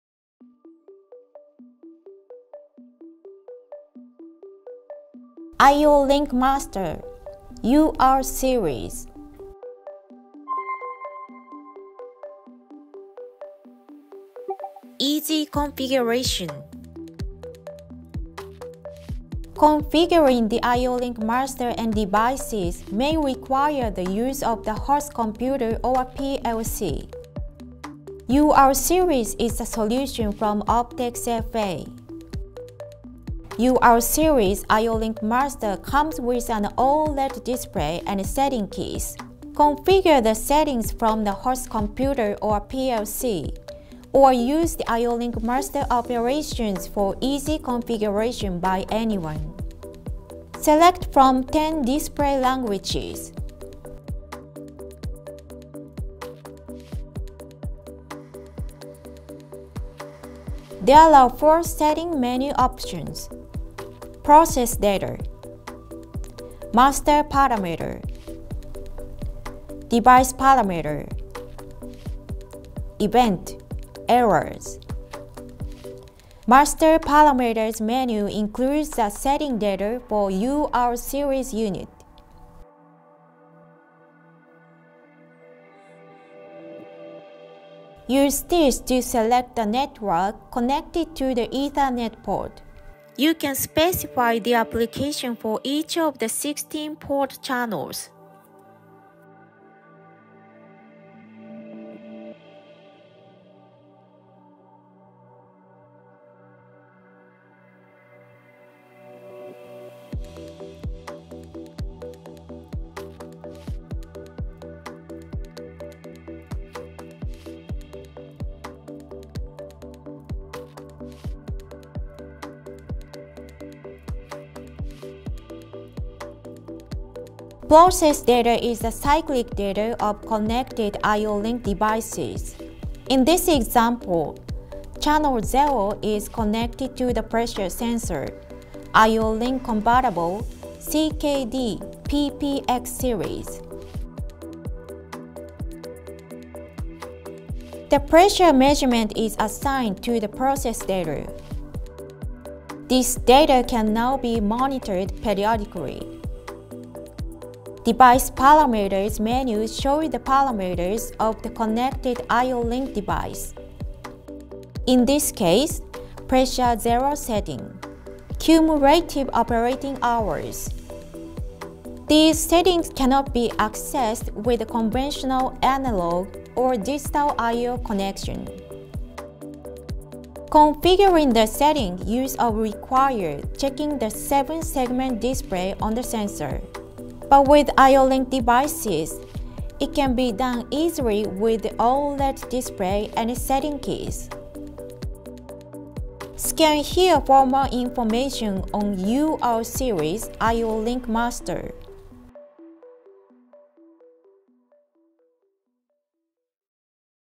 【UR Series】Easy configuration
Introducing the IO-Link Master "UR Series".
UR Series IO-Link Master comes with an OLED display and setting keys.
Configure the settings from a host computer or PLC, or use the IO-Link Master operations for easy configuration by anyone.
Select from 10 display languages.
Previous settings while checking the 7-segment display on the sensor itself can be easily done with the OLED display and setting keys for IO-Link devices.